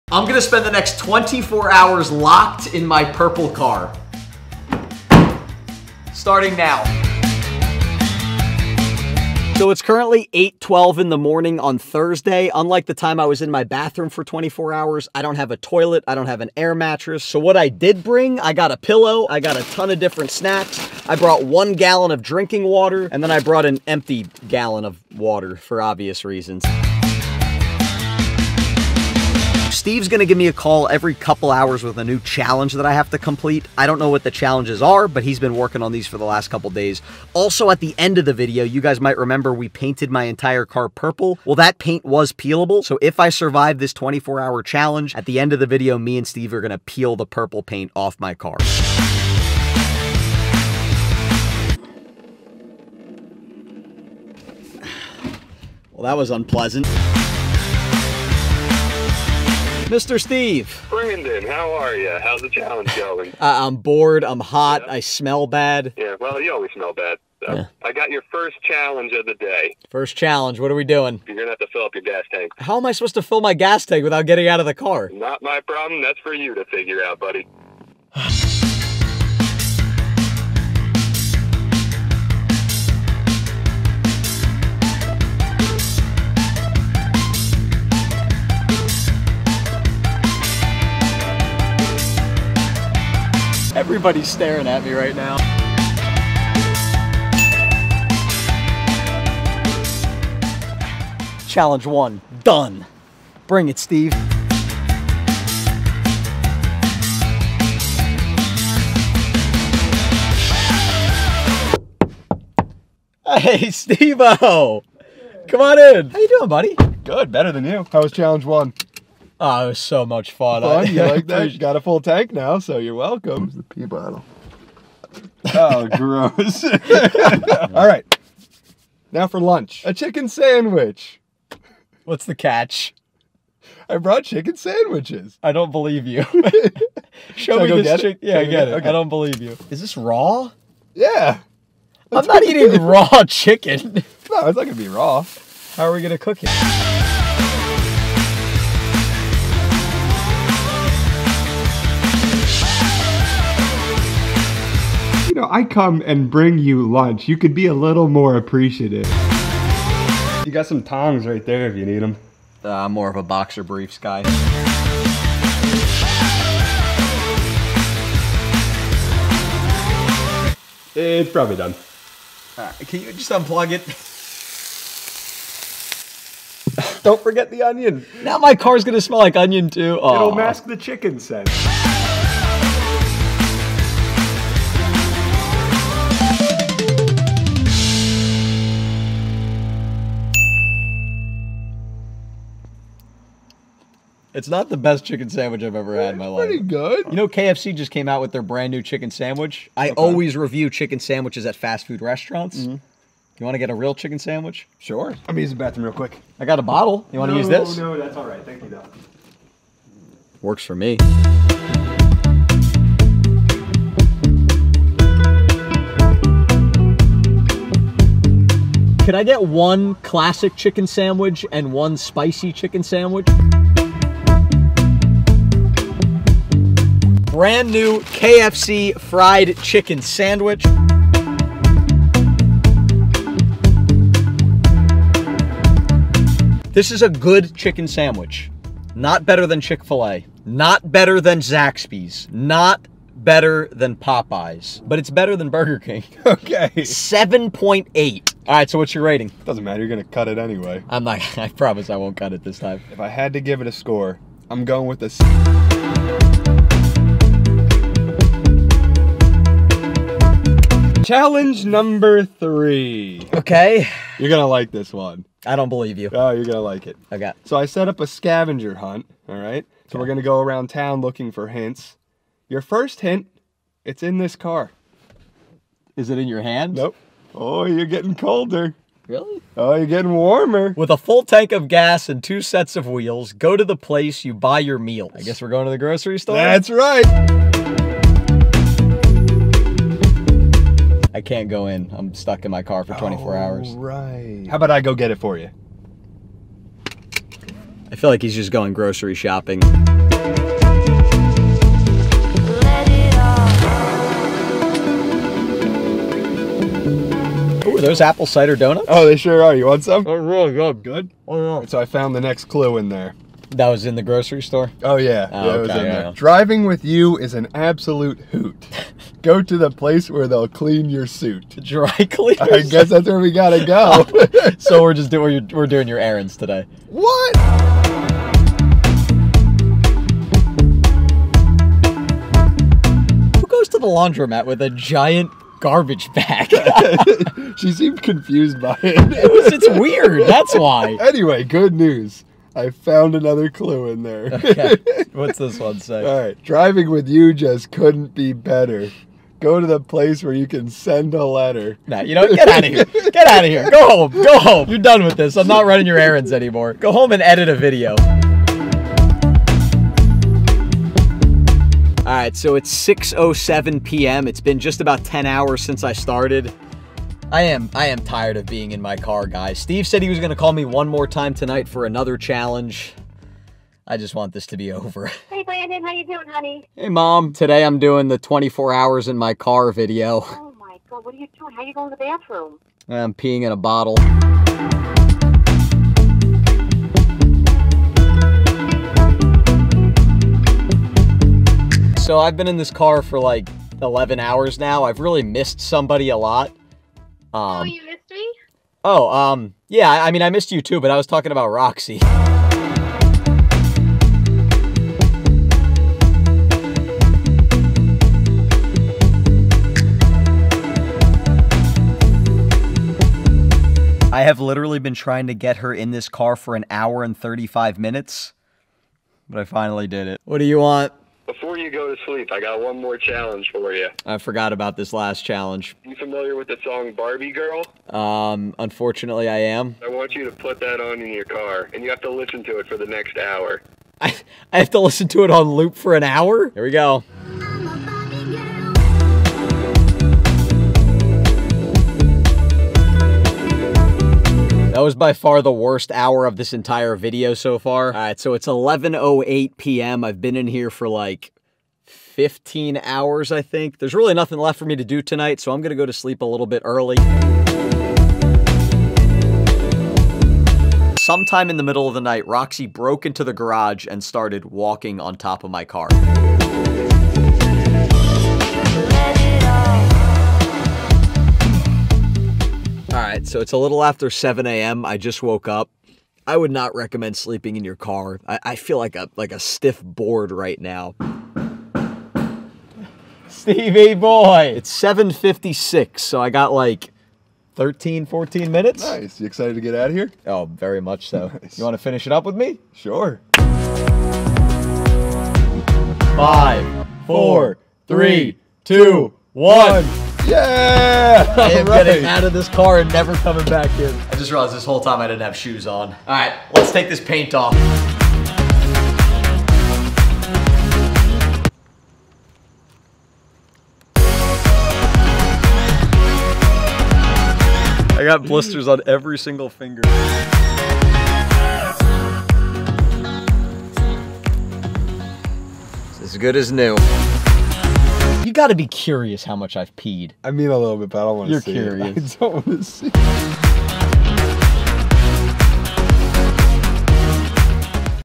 Locked In My Purple Car For 24 HOURS!
Locked In My Purple Car For 24 Hours | TFG Vlogs are back and today I will be locking myself in my car for a day! You have all been asking for me to take on a 24 hour video and today is that day! See if I can make it 24 hours in my car!